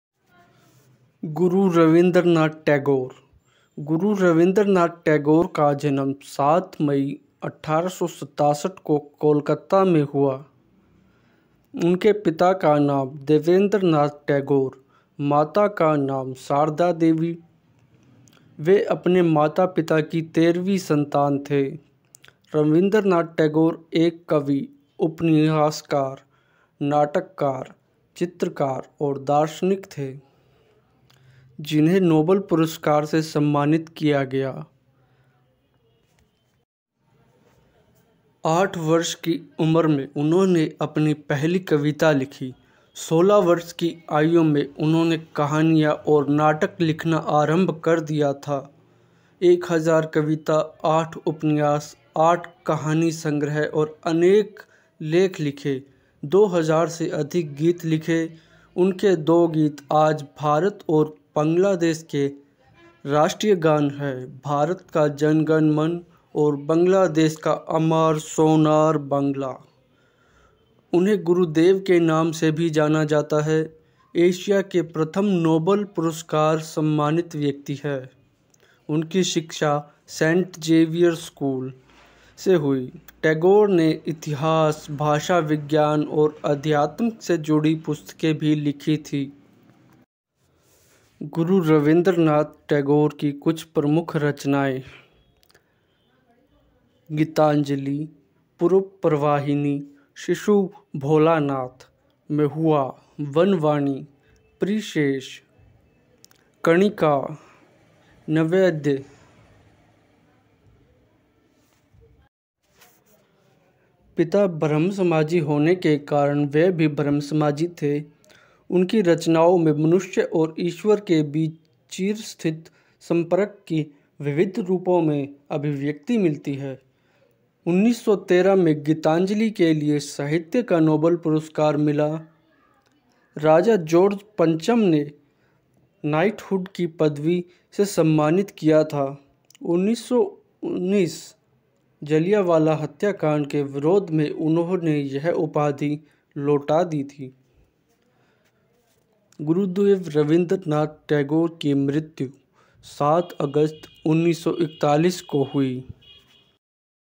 all about Rabindranath tagore in hindi. life of legendary people of world. history of golden era
simply click on my videos and enjoy  great story  .